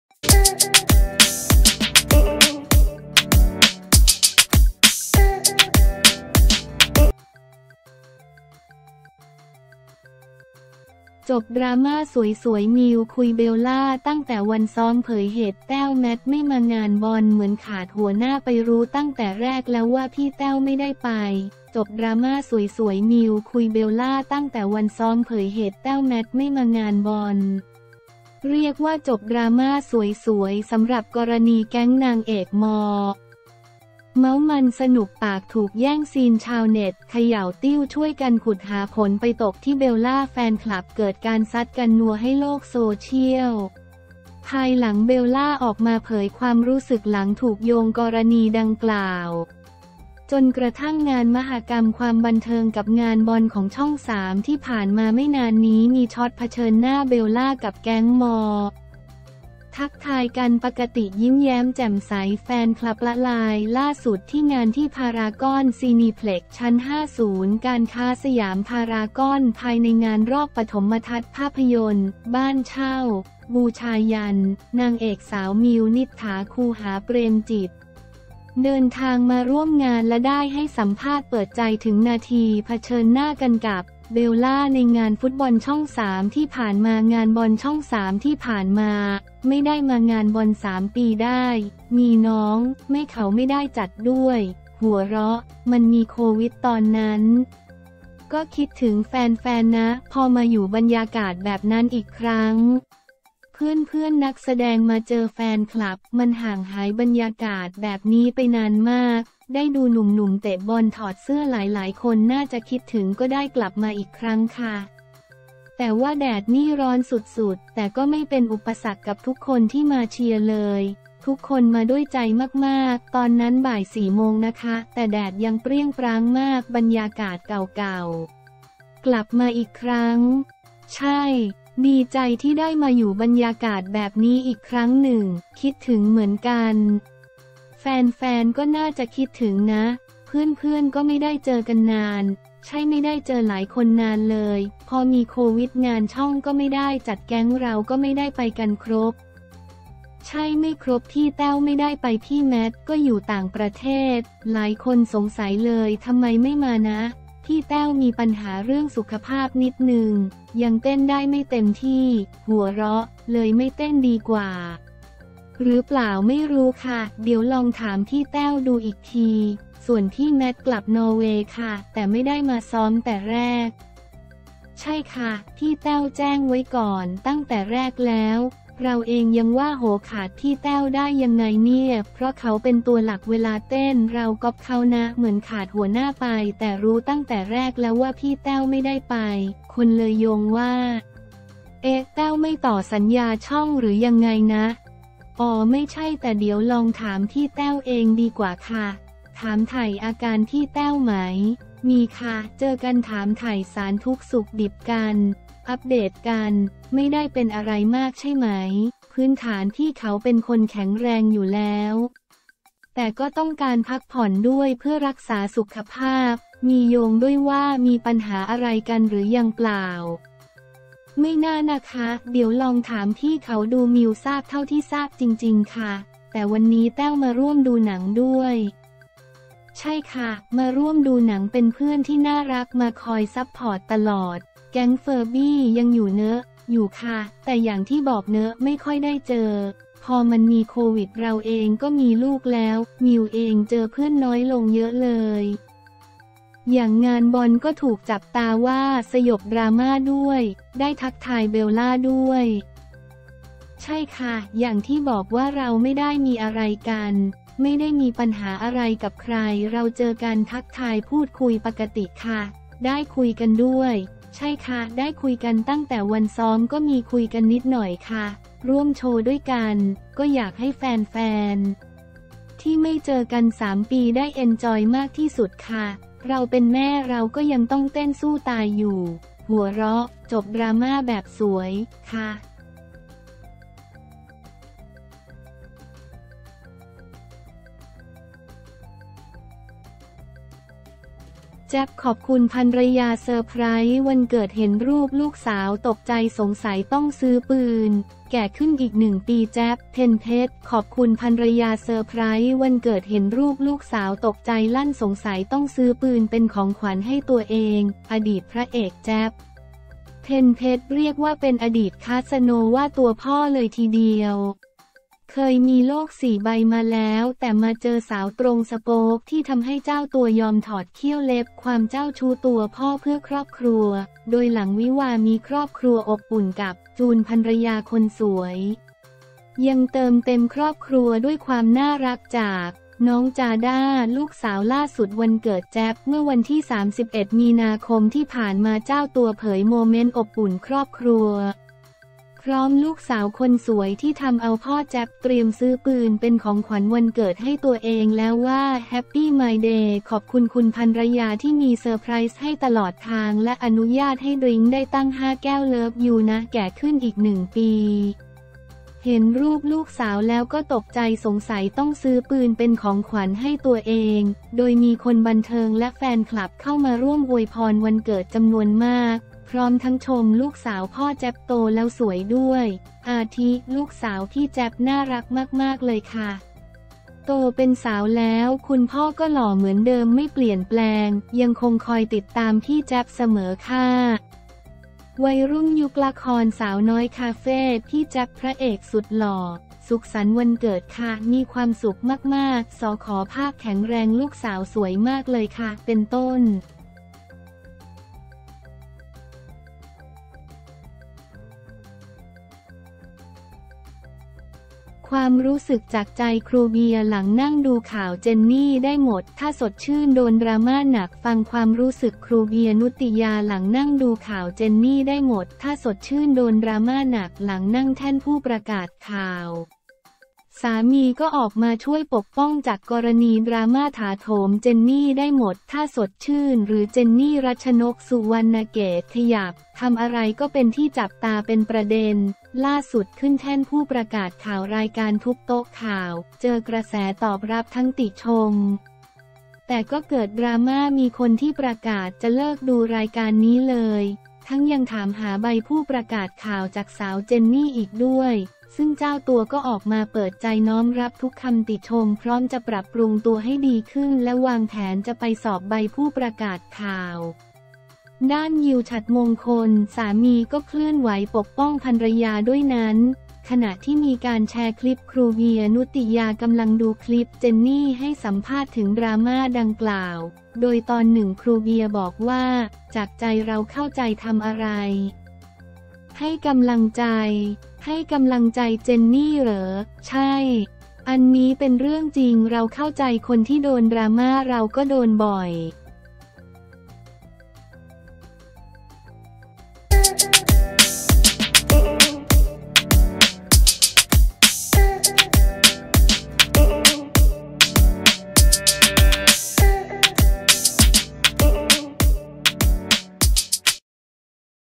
จบดราม่าสวยๆ มิว คุย เบลล่า ตั้งแต่วันซ้อม เผยเหตุ แต้ว แมท ไม่มางานบอล
เหมือนขาดหัวหน้าไป รู้ตั้งแต่แรกแล้วว่าพี่แต้วไม่ได้ไป จบดราม่าสวยๆ มิว คุย เบลล่า ตั้งแต่วันซ้อม เผยเหตุ แต้ว-แมท ไม่มางานบอล
เรียกว่าจบดราม่าสวยๆ สำหรับกรณีแก๊งนางเอก ม. เม้าธ์มันสนุกปาก ถูกแย่งซีน ชาวเน็ตเขย่าติ้ว ช่วยกันขุดหาผลไปตกที่ เบลล่า แฟนคลับเกิดการซัดกันนัวให้โลกโซเชียล ภายหลัง เบลล่า ออกมาเผยความรู้สึกหลังถูกโยงกรณีดังกล่าว
จนกระทั่งงานมหกรรมความบันเทิงกับงานบอลของช่อง 3 ที่ผ่านมาไม่นานนี้ มีช็อตเผชิญหน้า เบลล่ากับแก๊ง ม. ทักทายกันปกติ ยิ้มแย้มแจ่มใส แฟนคลับละลาย

ล่าสุดที่งานที่ พารากอน ซีนีเพล็กซ์ ชั้น 5 ศูนย์การค้าสยามพารากอน ภายในงานรอบปฐมทัศน์ ภาพยนตร์ “บ้านเช่า..บูชายัญ” นางเอกสาว มิว-นิษฐา คูหาเปรมจิต เดินทางมาร่วมงานและได้ให้สัมภาษณ์เปิดใจถึงนาทีเผชิญหน้ากันกับ เบลล่า ในงานฟุตบอลช่อง3 ที่ผ่านมา
งานบอลช่อง 3 ที่ผ่านมา?“ไม่ได้มางานบอล 3 ปีได้ (มีน้อง?) ไม่เขาไม่ได้จัดด้วย (หัวเราะ) มันมีโควิดตอนนั้น ก็คิดถึงแฟนๆ นะ พอมาอยู่บรรยากาศแบบนั้นอีกครั้ง เพื่อนๆ นักแสดง มาเจอแฟนคลับ มันห่างหายบรรยากาศแบบนี้ไปนานมาก ได้ดูหนุ่มๆ เตะบอล ถอดเสื้อ หลายๆ คนน่าจะคิดถึง ก็ได้กลับมาอีกครั้งค่ะ แต่ว่าแดดนี่ร้อนสุดๆ แต่ก็ไม่เป็นอุปสรรคกับทุกคนที่มาเชียร์เลย ทุกคนมาด้วยใจมากๆ ตอนนั้นบ่ายสี่โมงนะคะ แต่แดดยังเปรี้ยงปร้างมาก
บรรยากาศเก่าๆ กลับมาอีกครั้ง?
“ใช่ ดีใจที่ได้มาอยู่บรรยากาศแบบนี้อีกครั้งหนึ่ง คิดถึงเหมือนกัน แฟนๆ ก็น่าจะคิดถึงนะ (เพื่อนๆ ก็ไม่ได้เจอกันนาน?) ใช่ ไม่ได้เจอหลายคนนานเลย พอมีโควิดงานช่องก็ไม่ได้จัด
แก๊งเราก็ไม่ได้ไปกันครบ?
“ใช่ ไม่ครบ พี่แต้วไม่ได้ไป พี่แมทก็อยู่ต่างประเทศ (หลายคนสงสัยเลยทำไมไม่มานะ?) พี่แต้วมีปัญหาเรื่องสุขภาพนิดนึง ยังเต้นได้ไม่เต็มที่ (หัวเราะ) เลยไม่เต้นดีกว่า หรือเปล่าไม่รู้ค่ะเดี๋ยวลองถามพี่แต้วดูอีกที ส่วนพี่แมทกลับนอร์เวย์ค่ะ
แต่ไม่ได้มาซ้อมแต่แรก?
“ใช่ค่ะ พี่แต้วแจ้งไว้ก่อนตั้งแต่แรกแล้ว เราเองยังว่า โหขาดพี่แต้วได้ยังไงเนี่ย เพราะเขาเป็นตัวหลักเวลาเต้น เราก๊อปเค้านะ เหมือนขาดหัวหน้าไป แต่รู้ตั้งแต่แรกแล้วว่าพี่แต้วไม่ได้ไป”

คนเลยโยงว่า เอ๊ะแต้วไม่ต่อสัญญาช่องหรือยังไงนะ?
“อ๋อ ไม่ใช่ แต่เดี๋ยวลองถามพี่แต้วเองดีกว่าค่ะ (ถามไถ่อาการพี่แต้วไหม?) มีค่ะ เจอกันถามไถ่สารทุกข์สุขดิบกัน อัปเดตกัน (ไม่ได้เป็นอะไรมากใช่ไหม?) พื้นฐานพี่เขาเป็นคนแข็งแรงอยู่แล้ว แต่ก็ต้องการพักผ่อนด้วยเพื่อรักษาสุขภาพ”

มีโยงด้วยว่ามีปัญหาอะไรกันหรือยังเปล่า?
“ไม่น่านะคะ เดี๋ยวลองถามพี่เขาดู มิวทราบเท่าที่ทราบจริงๆค่ะ”

แต่วันนี้แต้วมาร่วมดูหนังด้วย?
“ใช่ค่ะ มาร่วมดูหนัง เป็นเพื่อนที่น่ารักมาคอยซัพพอร์ตตลอด” (แก๊งเฟอร์บี้ยังอยู่เนอะ?) อยู่ค่ะ แต่อย่างที่บอกเนอะไม่ค่อยได้เจอ พอมันมีโควิดเราเองก็มีลูกแล้ว มิวเองเจอเพื่อนน้อยลงเยอะเลย
อย่างงานบอลก็ถูกจับตาว่าสยบดราม่าด้วย ได้ทักทาย เบลล่า ด้วย?
“ใช่ค่ะ อย่างที่บอกว่าเราไม่ได้มีอะไรกัน ไม่ได้มีปัญหาอะไรกับใคร เราเจอกันทักทายพูดคุยปกติค่ะ (ได้คุยกันด้วย?) ใช่ค่ะได้คุยกัน ตั้งแต่วันซ้อมก็มีคุยกันนิดหน่อยค่ะ (ร่วมโชว์ด้วยกัน?) ก็อยากให้แฟนๆ ที่ไม่เจอกัน 3 ปี ได้เอ็นจอยมากที่สุดค่ะ เราเป็นแม่เราก็ยังต้องเต้นสู้ตายอยู่ (หัวเราะ) (จบดราม่าแบบสวย?) ค่ะ”